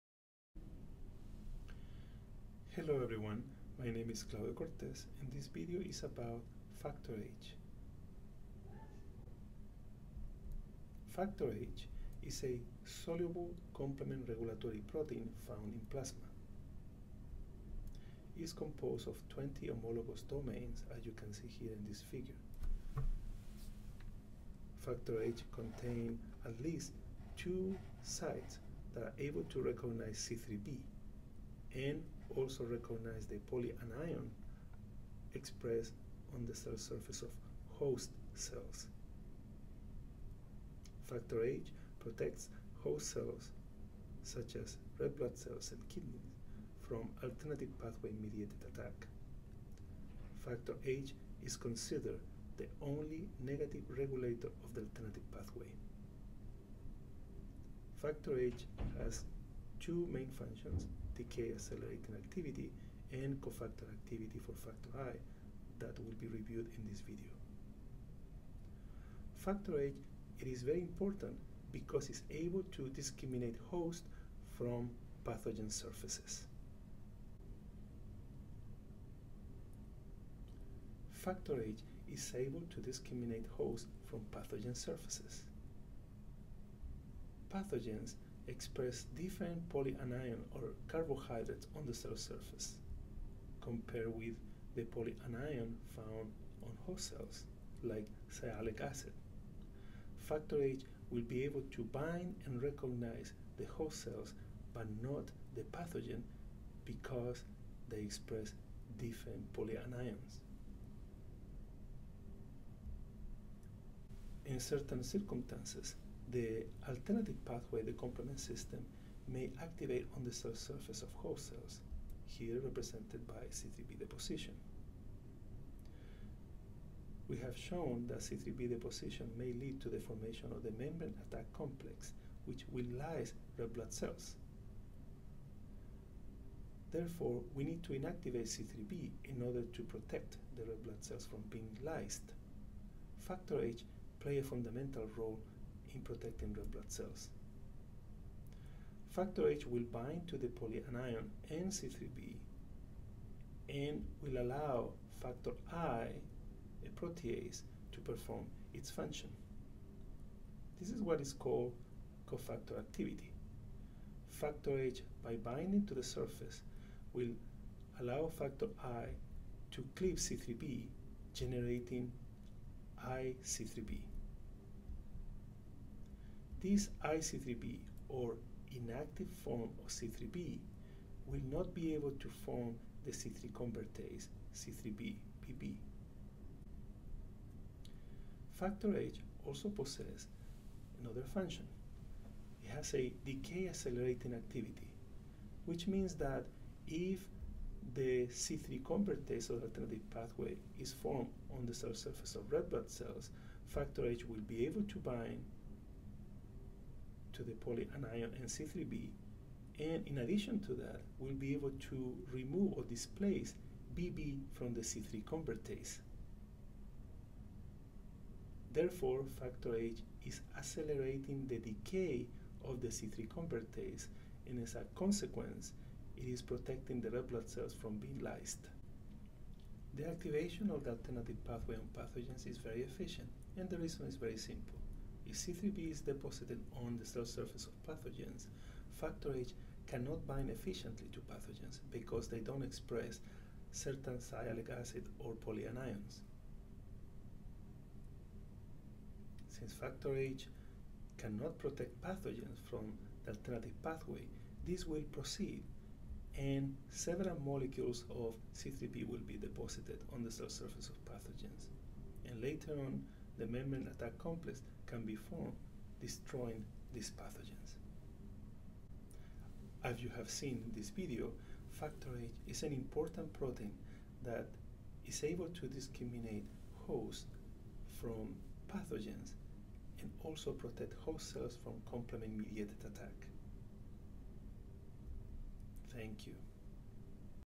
Factor H
Table of Contents:

00:02 - Factor H
00:11 - Characteristics of factor H
01:20 - Factor H discriminate host cells vs pathogens
01:56 - Function: Factor H cofactor for factor I
03:07 - Function: Factor H decay accelerating activity
03:49 - Factor H does not bind to pathogens allowing AP activation on pathogens